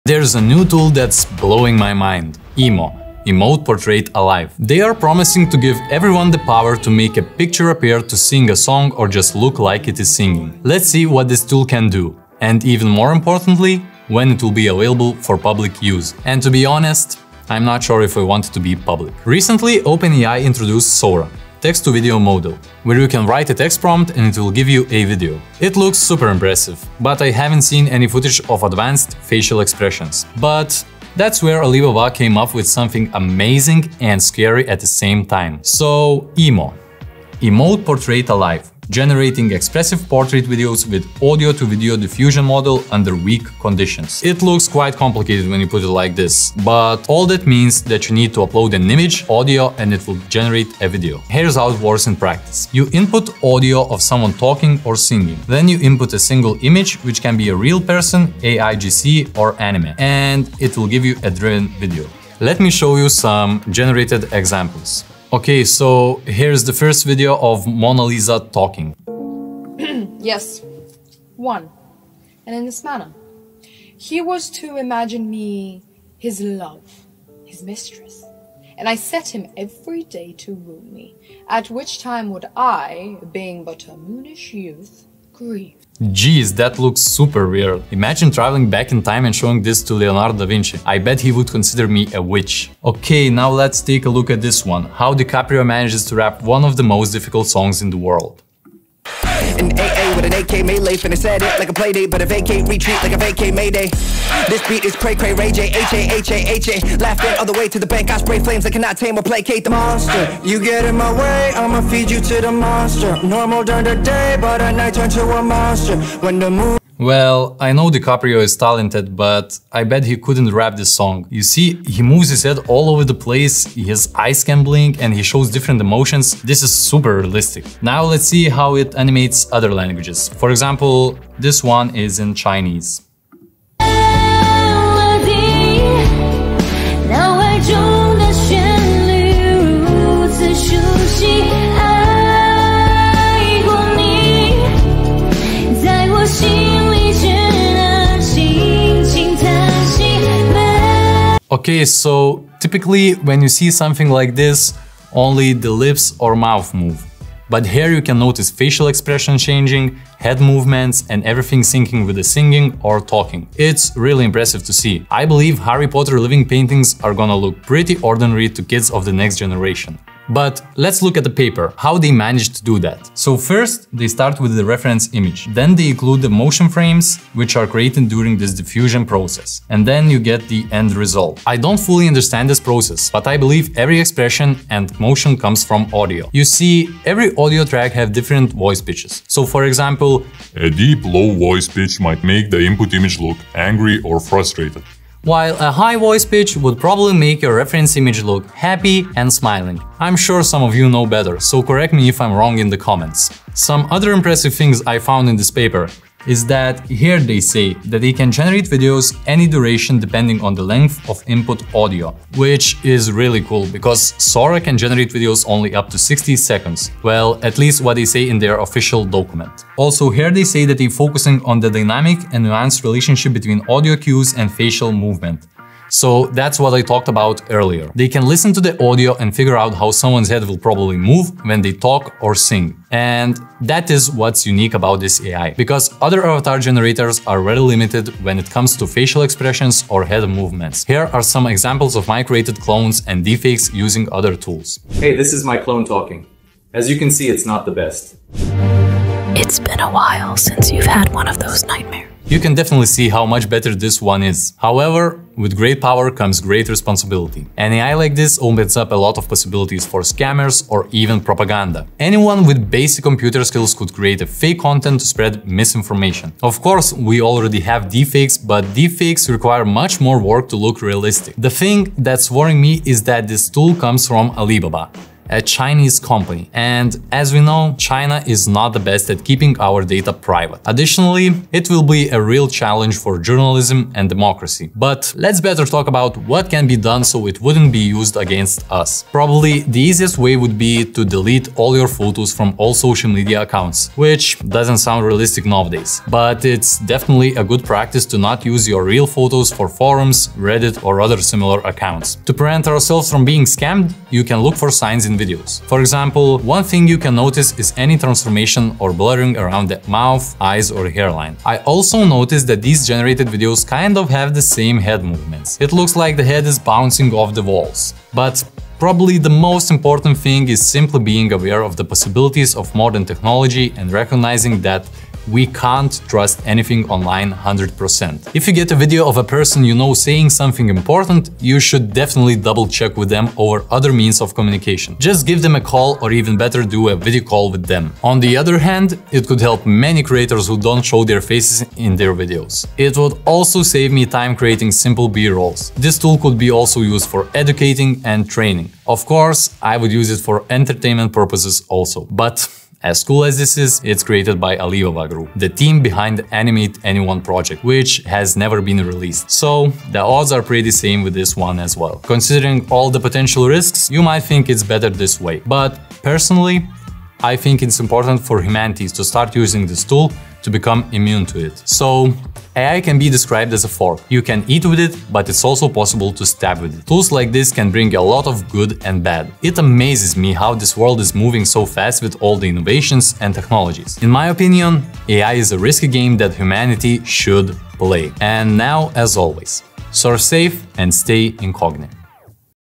I wish this AI tool never existed. Sora is dead?
Discover EMO: Emote Portrait Alive, a groundbreaking tool by Alibaba, which brings images to life. How does it work? Can you call it a bigger thing than Sora? What privacy concerns does it raise? What advantages does it offer? Find out in the video.

CHAPTERS:
00:00 Intro
00:35 EMO: Emote Portrait Alive
01:03 Examples
02:51 How does it work?
04:14 Dangers
04:45 How to become immune to it?
05:38 Advantages

Video made by Incogni channel.